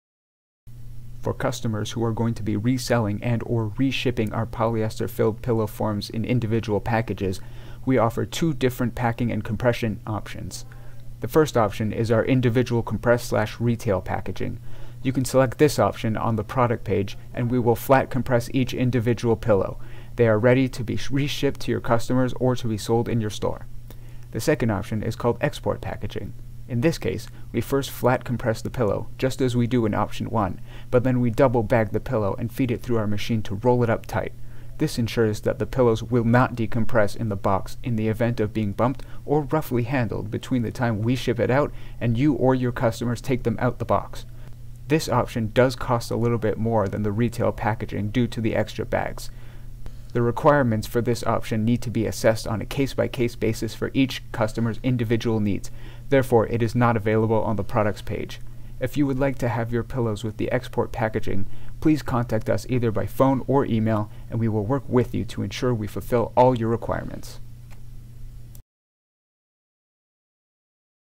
Options for Individually Compressed Pillow Forms
Flat-compressed and express packaging options for individually compressed polyester filled pillow forms.